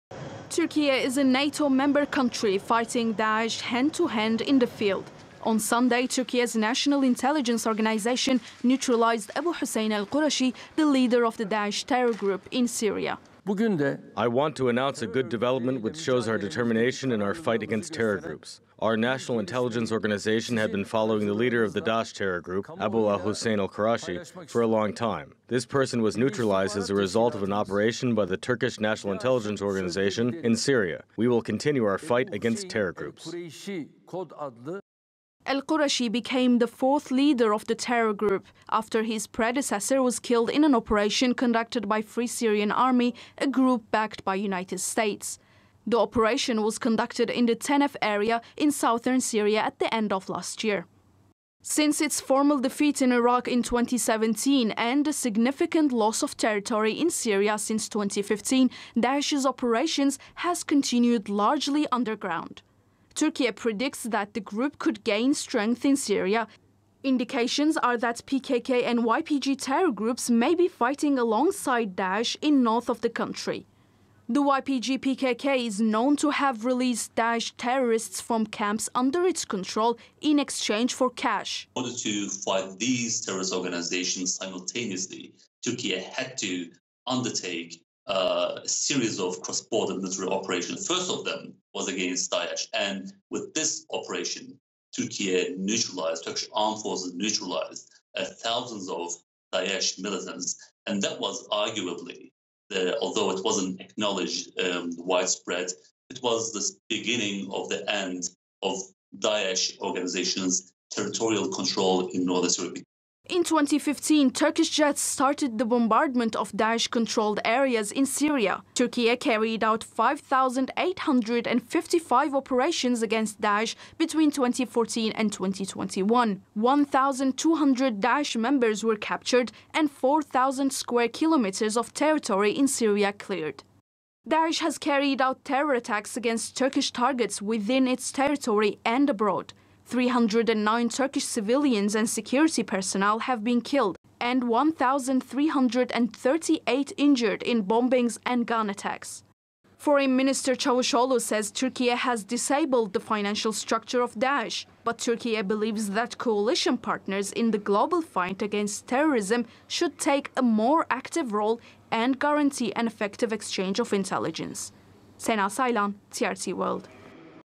Daesh leader neutralised by the Turkish forces during operation in Syria
Since 2015, Turkiye has been working to expose and destroy Daesh in Syria and Iraq. Late last month, police arrested 19 foreign nationals in Ankara who had allegedly been in contact with Daesh terrorists. On Sunday President Erdogan announced that the Turkish intelligence agency had neutralised Daesh leader Abu Hussein al-Qurashi during an operation in Syria. Sena Saylan reports.